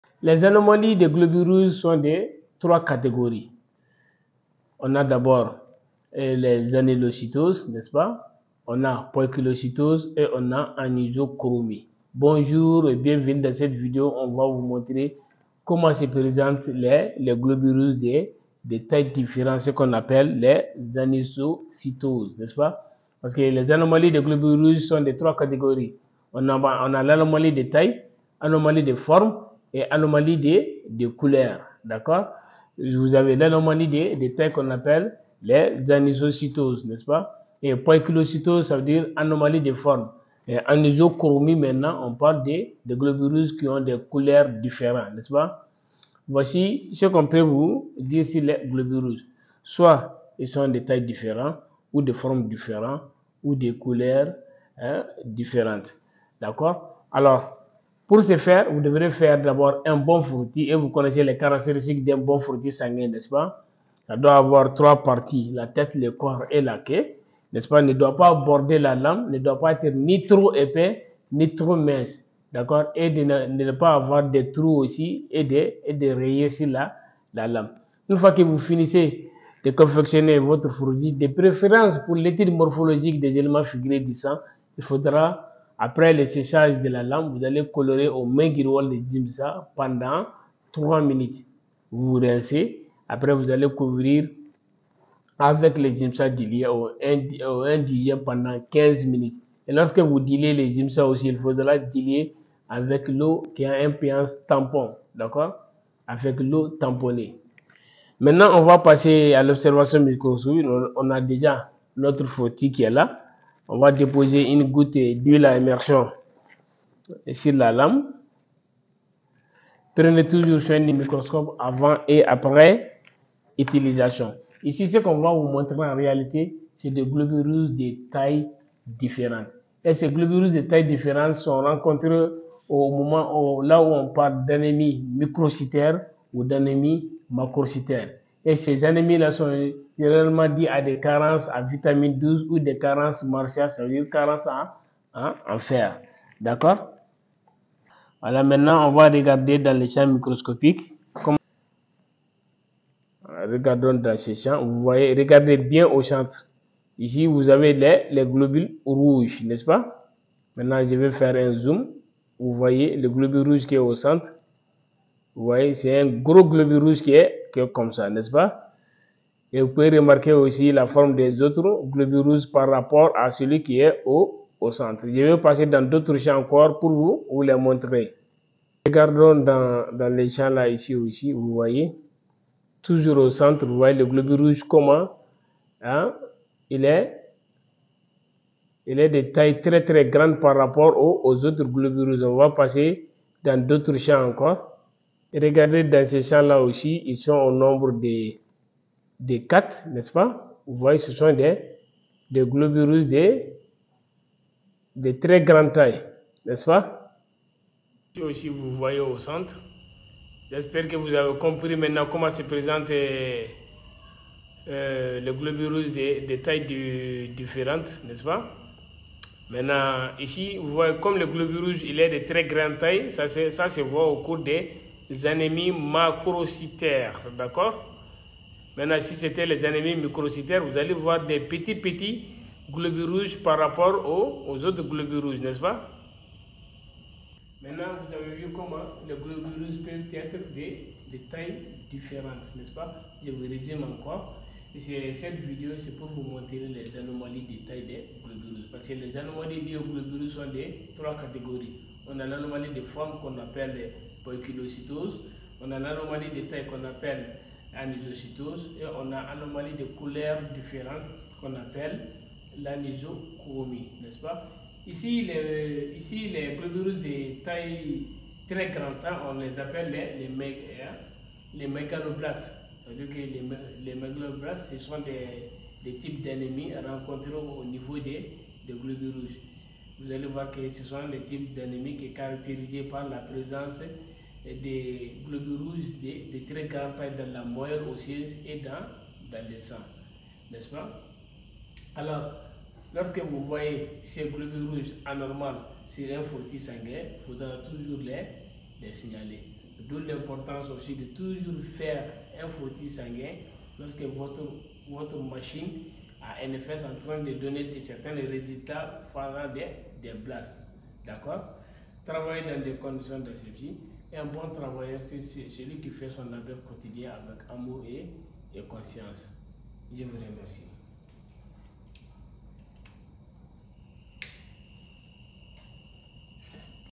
Les Anomalies de taille des Globules rouges sur un frottis sanguin ( L'anisocytose )
L'anisocytose est un terme médical qui désigne une variation anormale de la taille des globules rouges dans le sang. Normalement, les globules rouges ont une taille assez uniforme, mais en cas d'anisocytose, certains peuvent être plus grands ou plus petits que la normale. Cette condition peut être détectée par un test sanguin, notamment dans le cadre d'une numération formule sanguine (NFS).